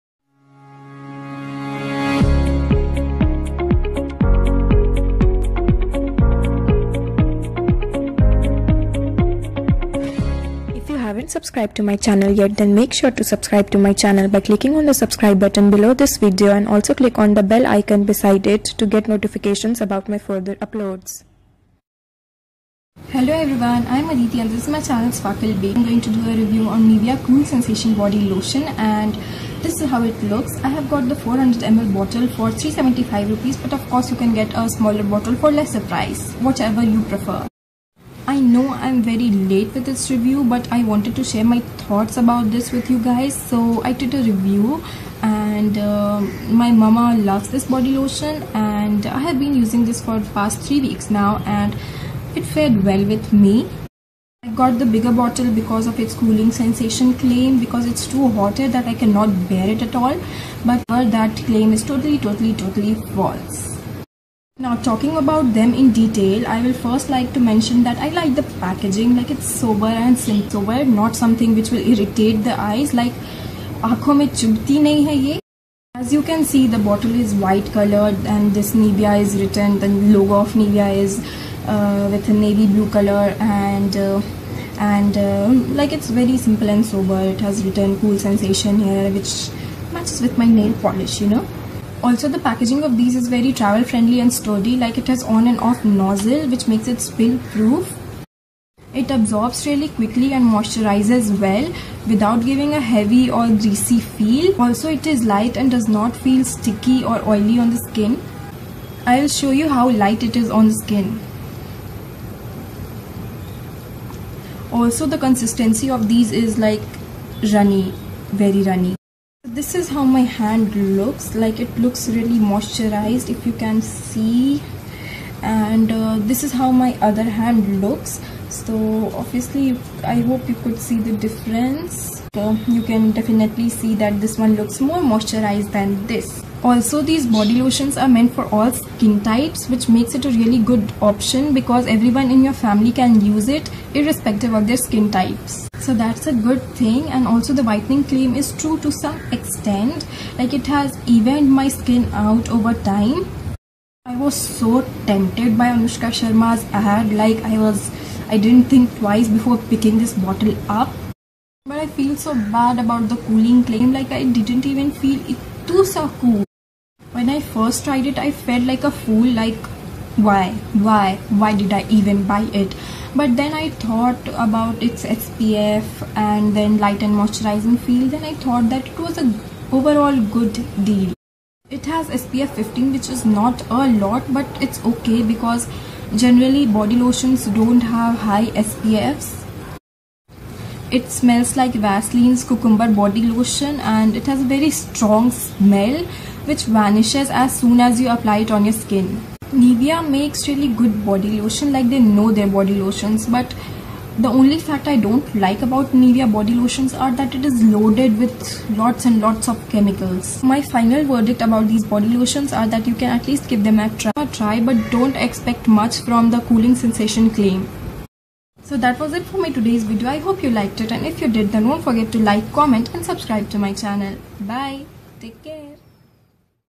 Nivea Cool Sensation Whitening Body Lotion Review | Worth or Not
Hello everyone,
Today I am going to review Nivea Cool Sensation Whitening body lotion.

Love,
Adity